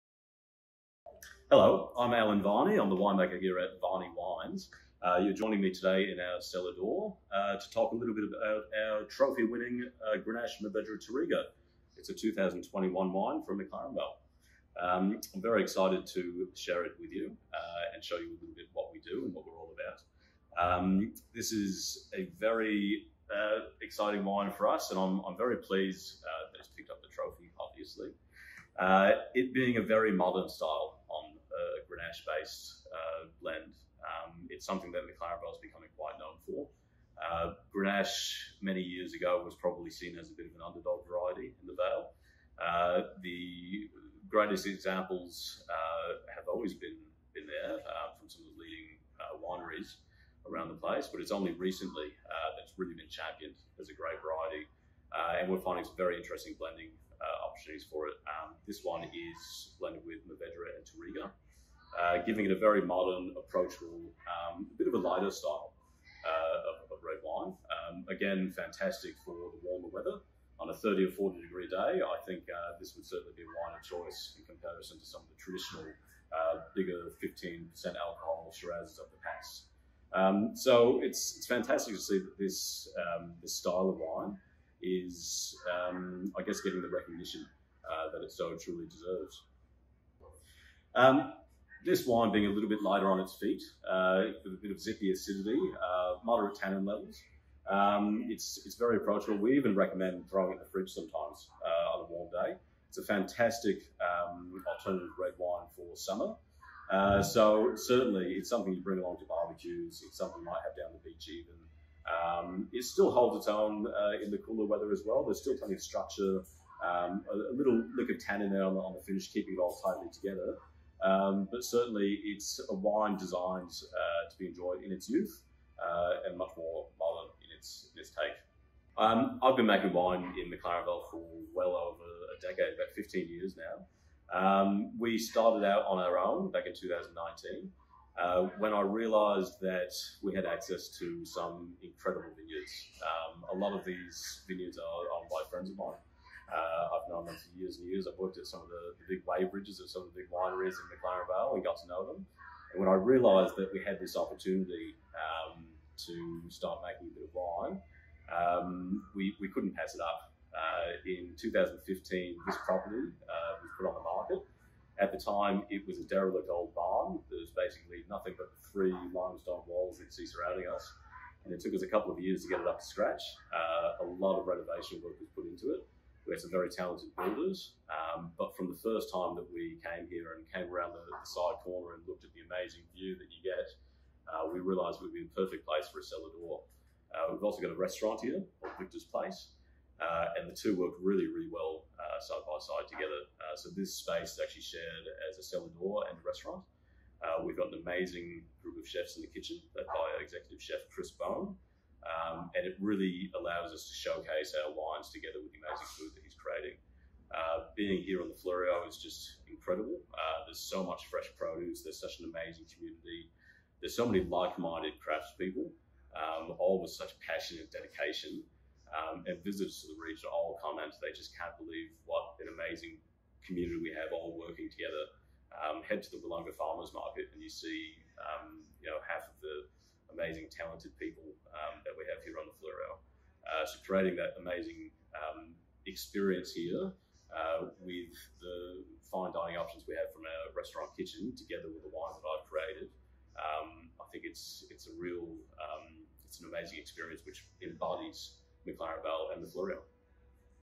2022 McLaren Vale Wine Show - Trophy Winning Wine Trail Stop 1
The Best Grenache Predominant Blend in the 2022 McLaren Vale Wine Show was awarded to Varney Wines 2021 Entrada Grenache Mourvedre Touriga. We caught up with Winemaker Alan Varney to gain a little more insight on this trophy winning wine, his love of McLaren Vale and the offering at Varney Wines Cellar Door.